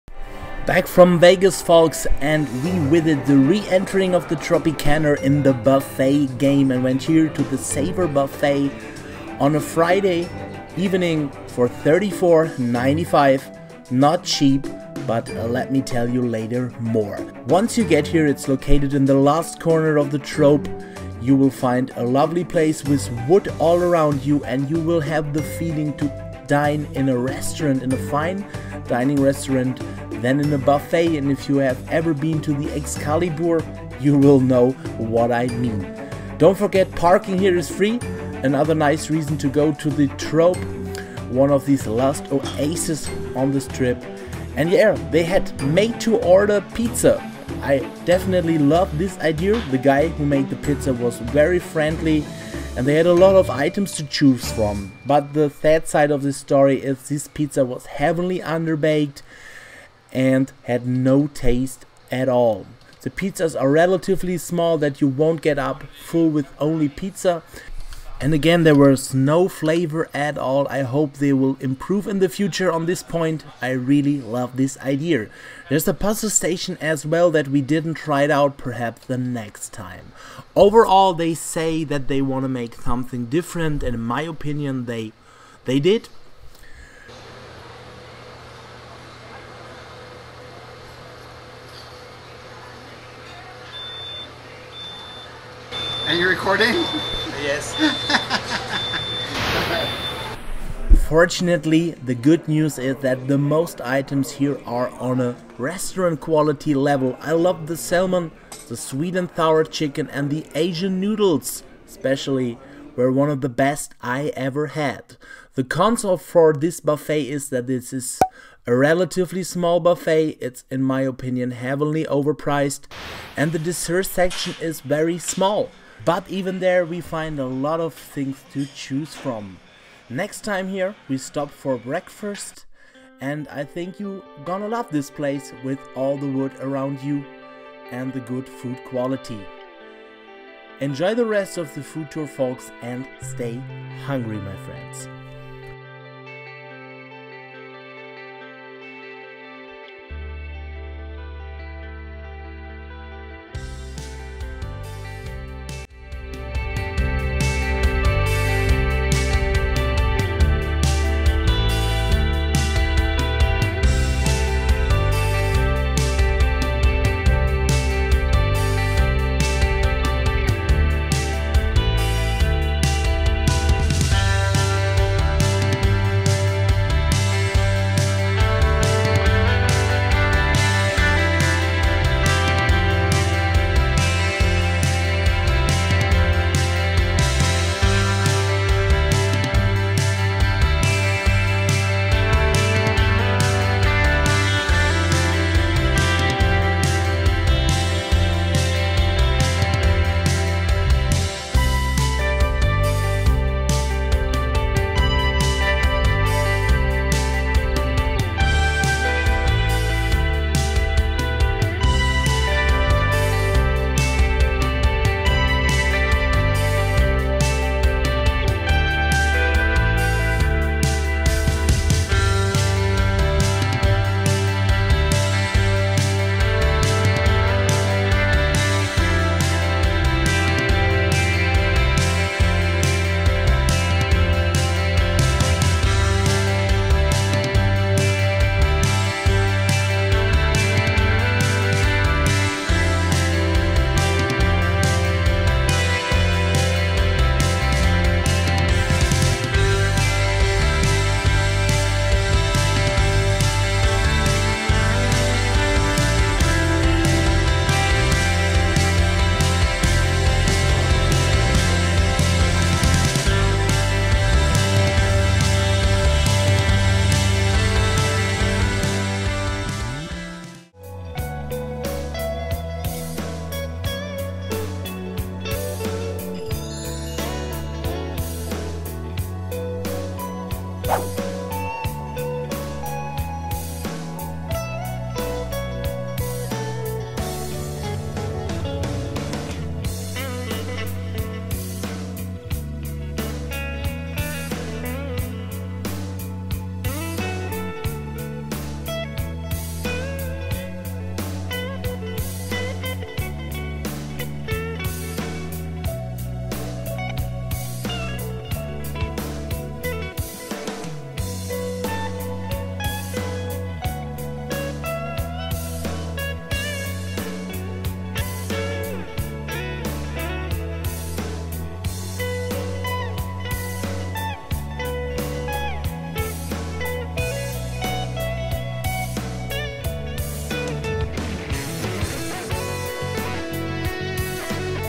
A new Buffet to Vegas: Tropicana - Savor The Buffet (Full Tour)
Follow me through this brand new Buffet (Dinner) Las Vegas (2017), the Savor Buffet located inside the legendary Tropicana.

Parking is free!